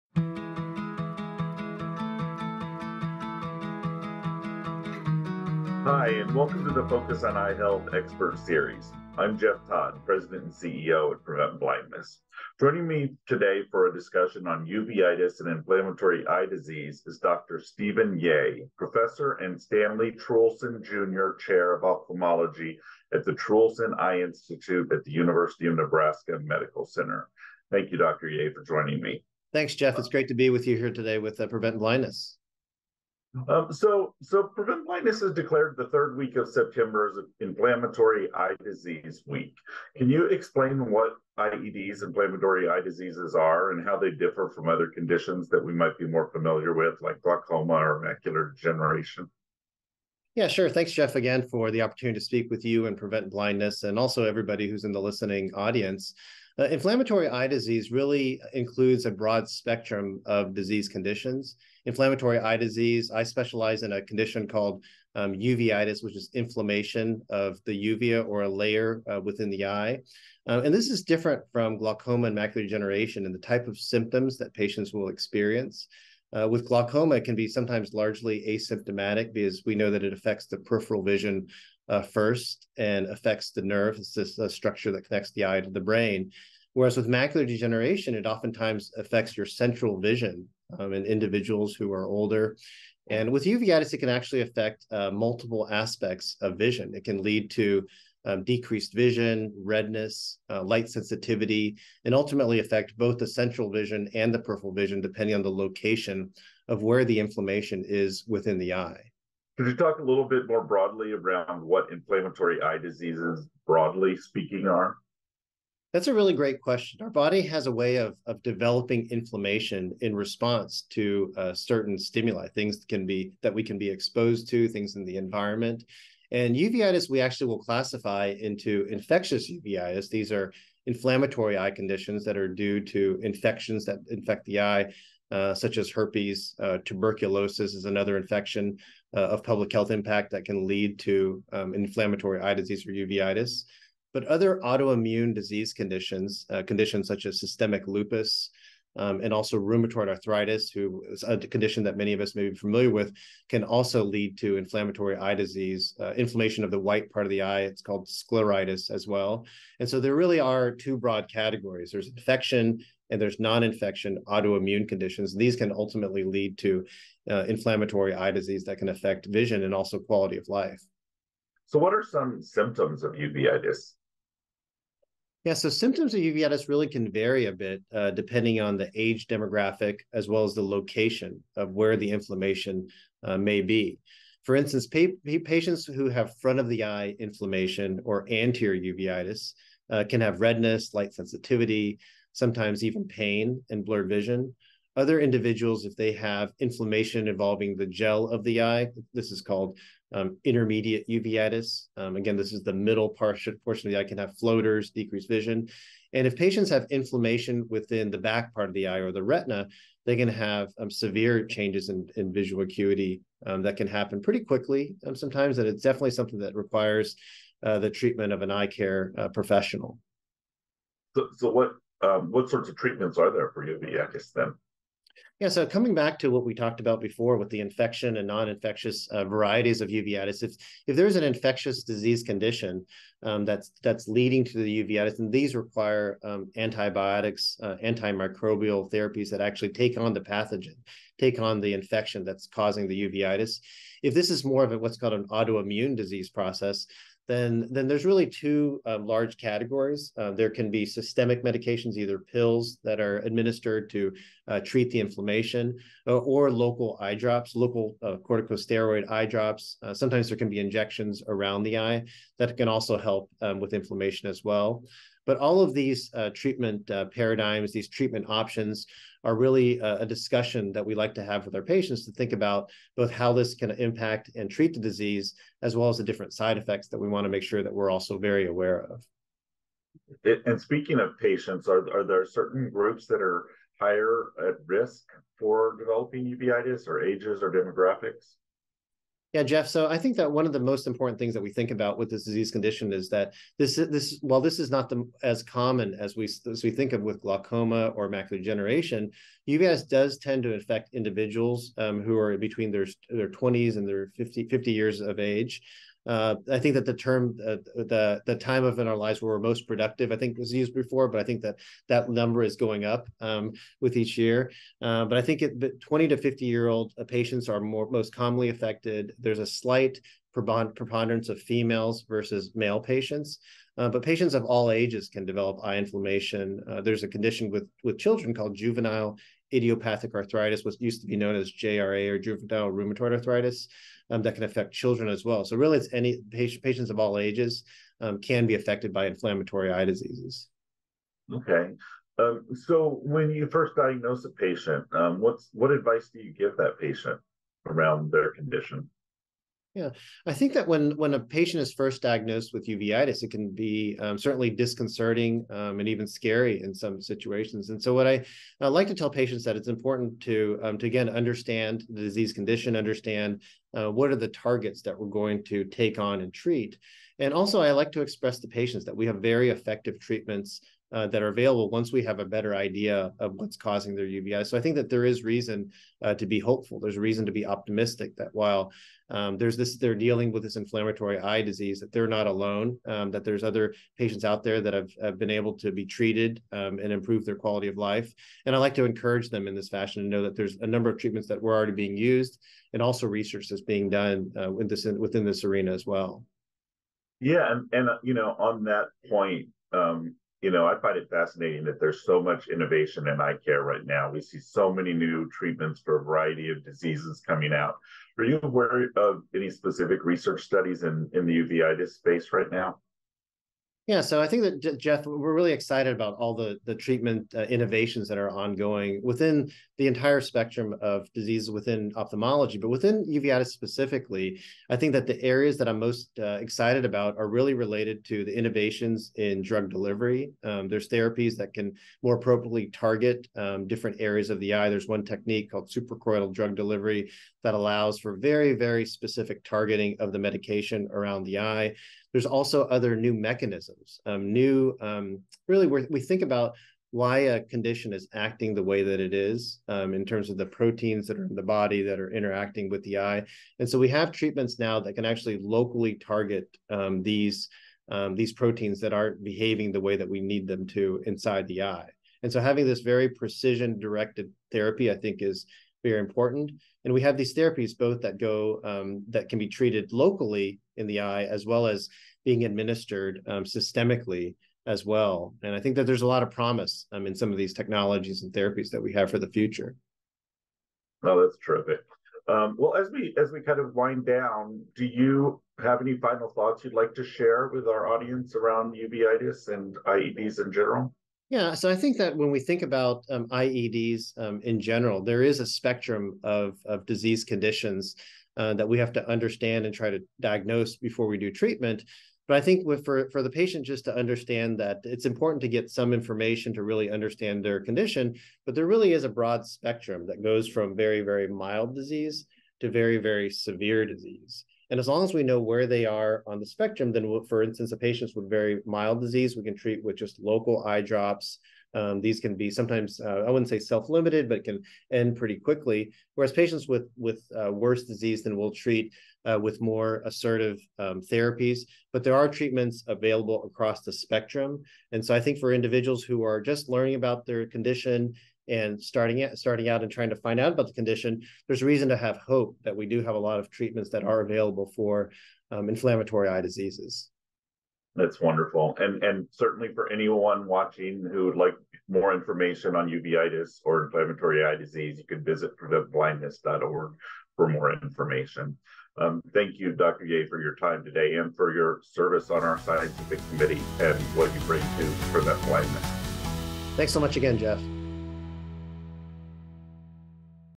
Uveitis and Inflammatory Eye Disease
Prevent Blindness president & CEO, Jeff Todd, discusses uveitis and inflammatory eye disease with uveitis expert Dr. Steven Yeh, Professor and Stanley Truhlsen Jr. Chair in Ophthalmology, Truhlsen Eye Institute, University of Nebraska Medical Center.